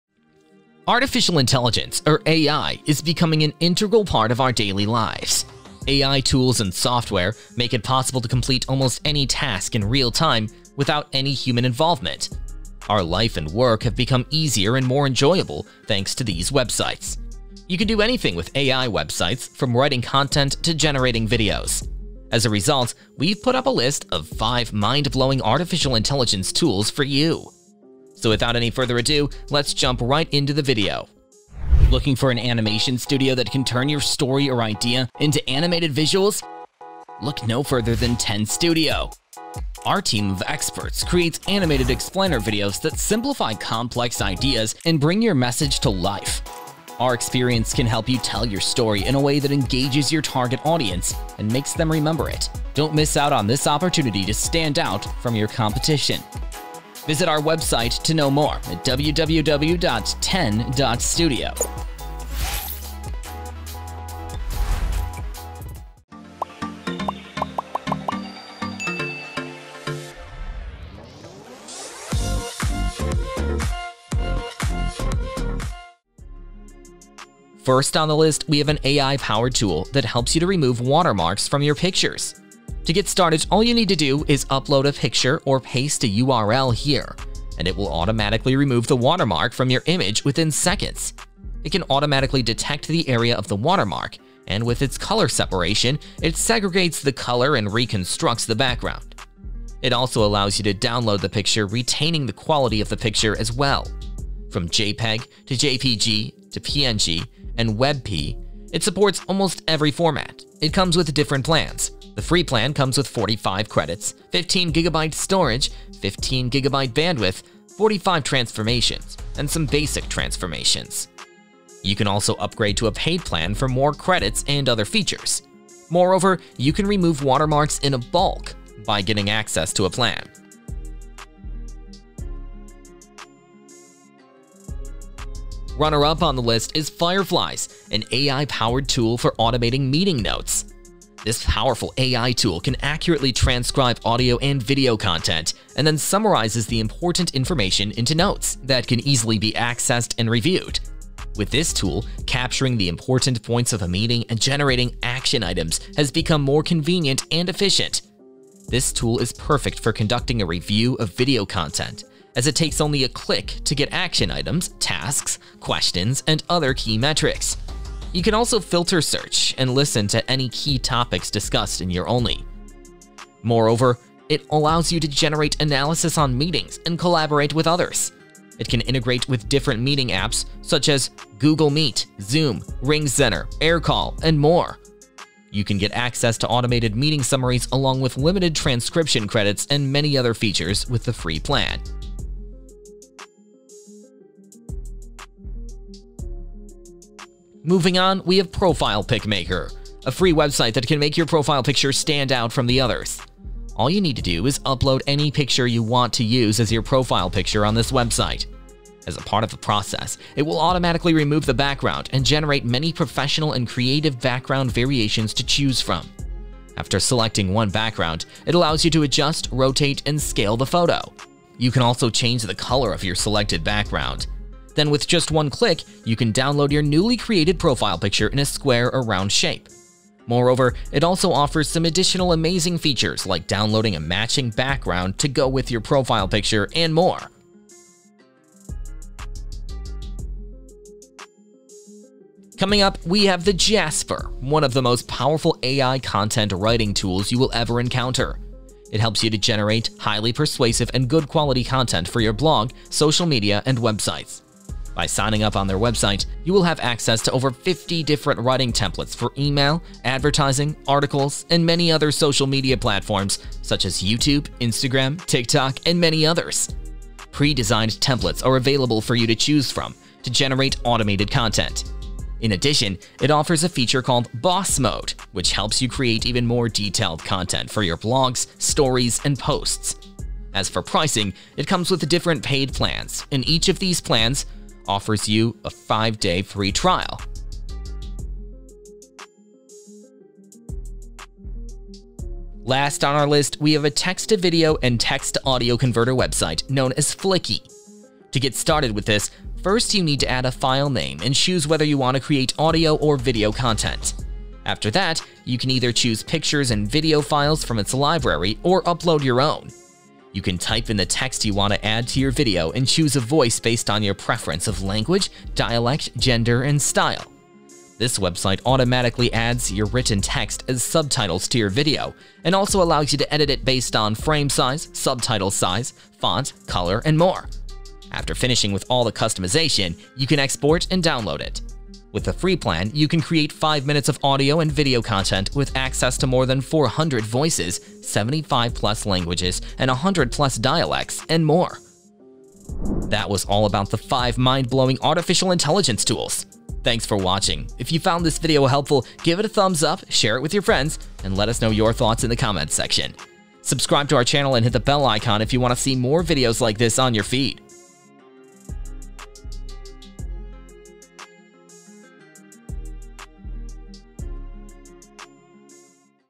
5 Mind blowing Artificial Intelligence Tools ▶5
Here are 5 mind-blowing AI tools that vary in terms of functionality, usefulness, and results. However, among all the available tools out there, finding the best one can be overwhelming.  With that in mind, here is our list of 5 mind-blowing Artificial intelligence tools.

5 Mind blowing Artificial Intelligence Tools

Watermark remover

Fireflies

Profile pic maker

Jasper

Fliki
Intro and Animation Credit:
10Studio: